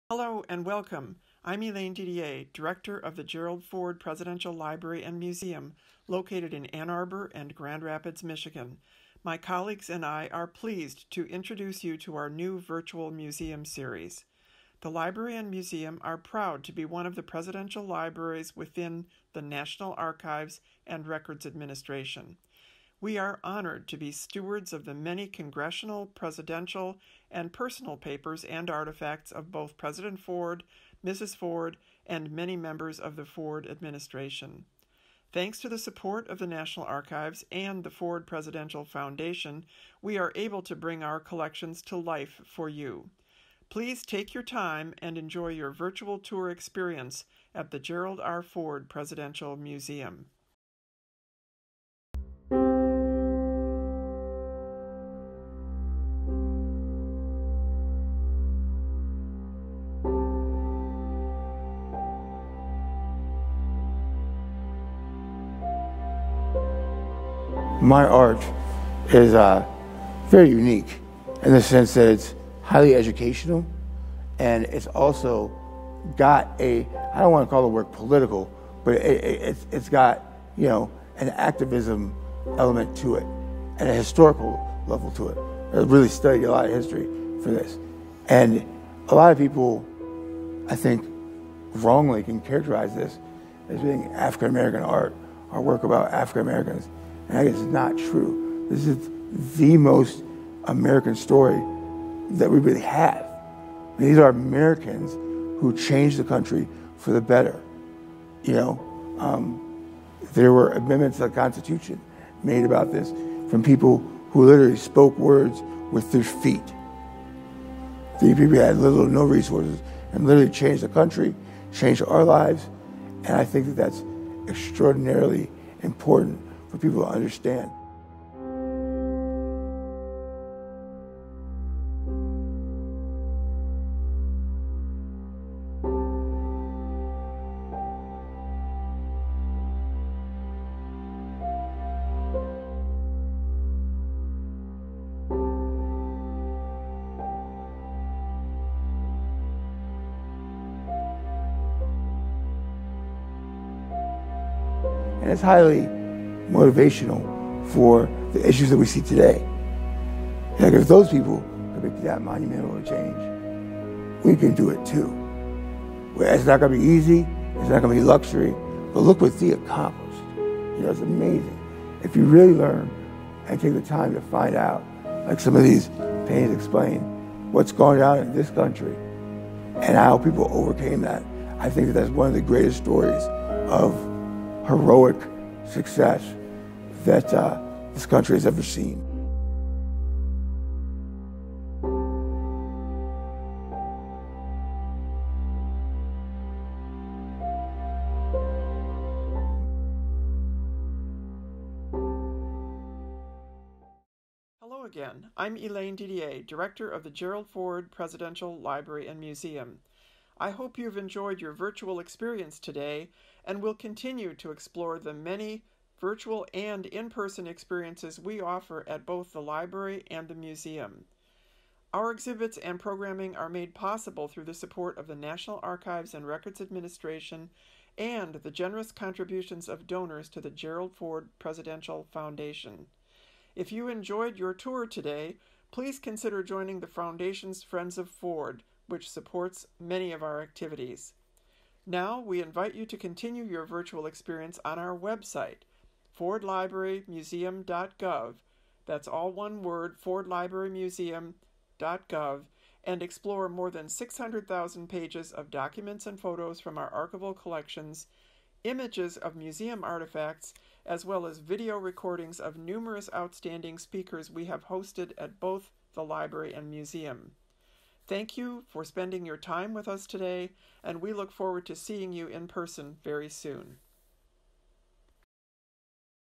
The Continual Struggle: The American Freedom Movement and the Seeds of Social Change
Hear from the artist Brian Washington as he introduces his artwork documenting the Civil Rights Movement and America’s historical struggle against segregation and other race-based injustices. The exhibit opened in February in honor of Black History Month, which President Ford instituted in 1976.